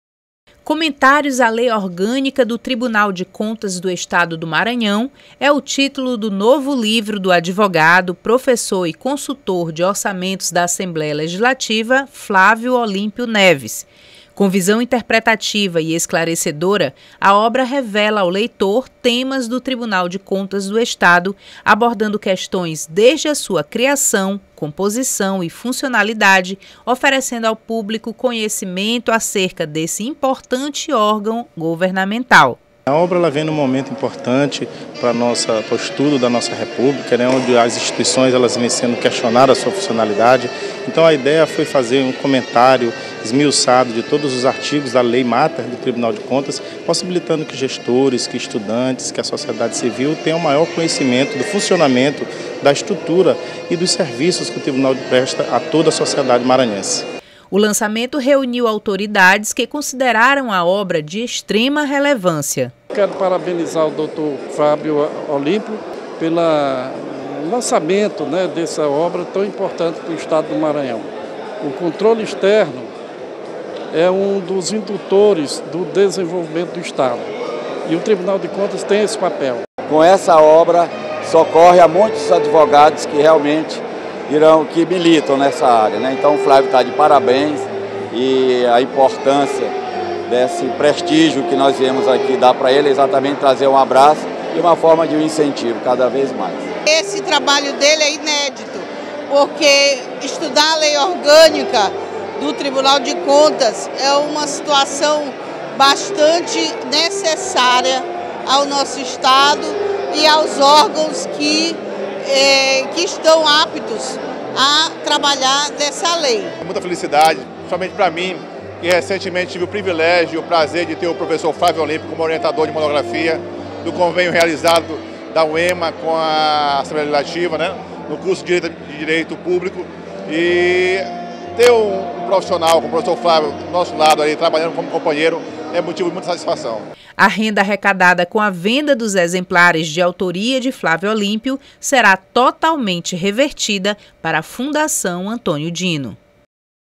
Flávio Olímpio lança livro sobre a Lei Orgânica do Tribunal de Contas do Maranhão
O consultor da Assembleia Legislativa do Maranhão, Flávio Olímpio Neves Silva, lançou, na noite de terça-feira (8), o livro “Comentários à Lei Orgânica do Tribunal de Contas do Estado do Maranhão”. O evento contou com a presença do presidente do TCE-MA, Washington Luiz de Oliveira; do vice-presidente do Tribunal Regional do Trabalho da 16ª Região, desembargador Gerson de Oliveira Costa Filho; do presidente da OAB/MA, Kaio Saraiva; familiares e amigos do autor.